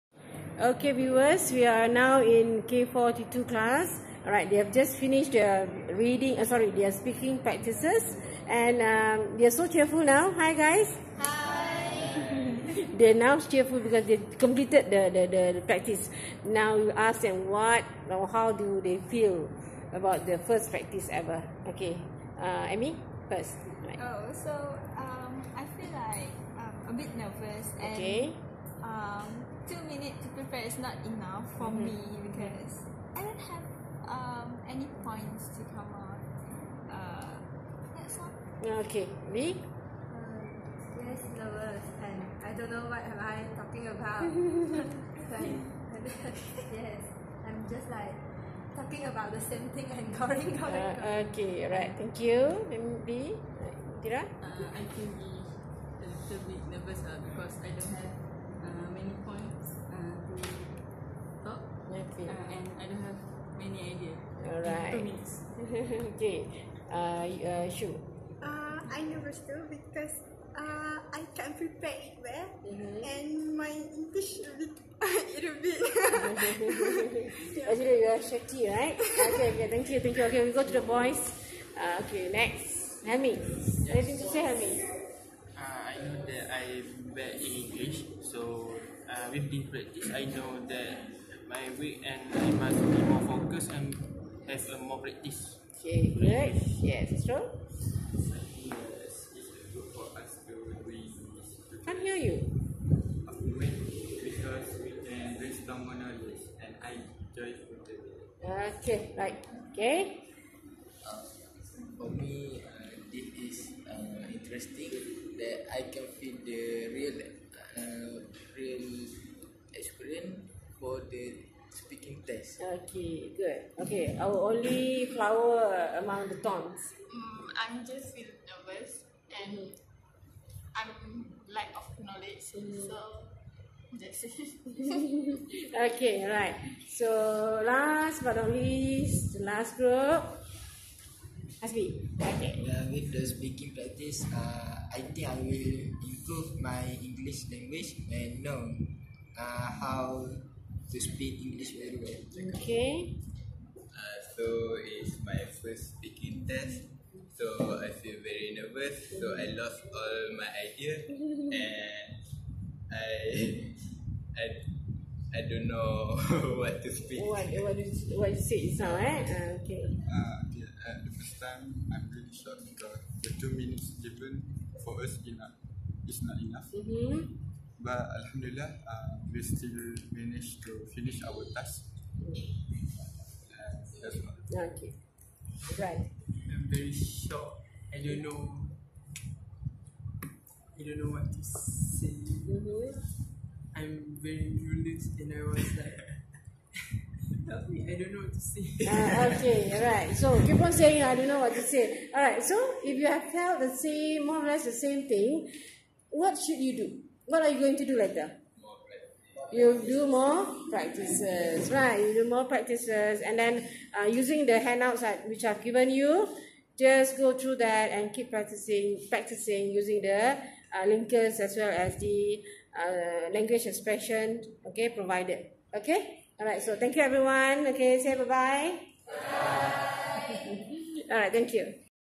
SPEAKING:POURING OUT THEIR HEARTS K4T2
They are expressing themselves after their first speaking practice.